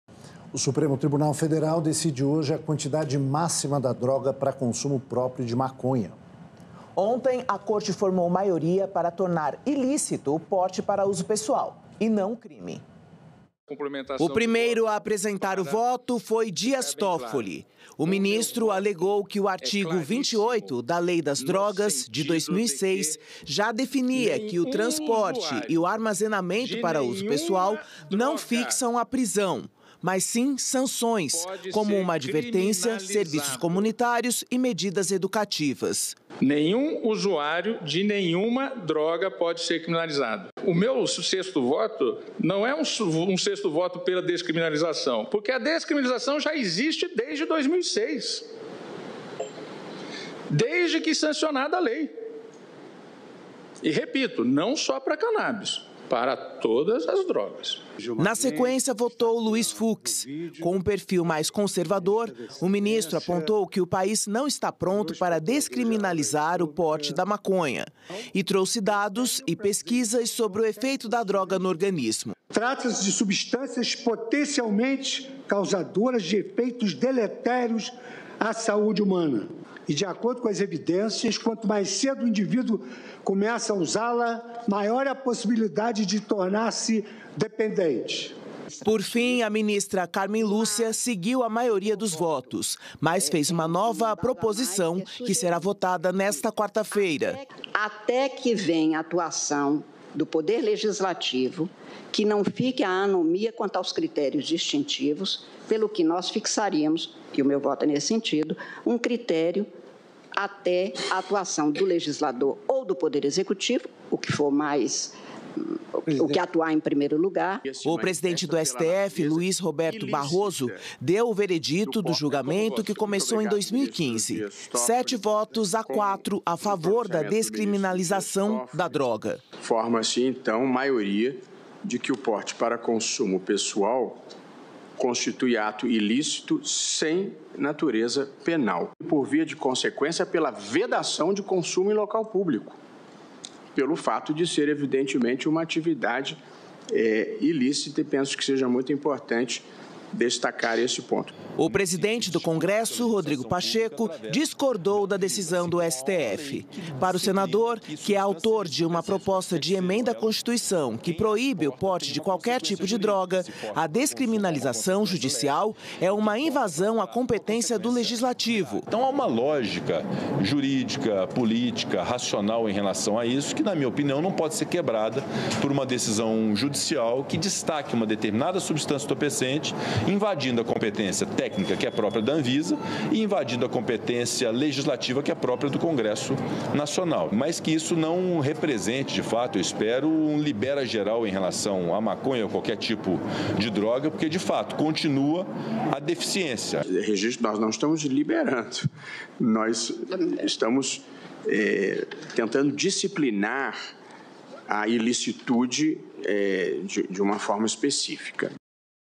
STF: quantidade de maconha permitida para consumo próprio será definido nesta quarta (26)
O presidente do STF, Luís Roberto Barroso, deu o veredicto do julgamento que começou em 2015, com sete votos a quatro a favor da descriminalização da cannabis.

Barroso afirmou que a maioria decidiu que o porte para consumo pessoal constitui um ato ilícito sem natureza penal e, consequentemente, o consumo em local público é vedado por ser uma atividade ilícita, destacando a importância desse ponto.

O presidente do Congresso, Rodrigo Pacheco, discordou da decisão do STF. Para o senador, que é autor de uma proposta de emenda à constituição que proíbe o porte de qualquer tipo de droga, a descriminalização judicial é uma invasão à competência do legislativo.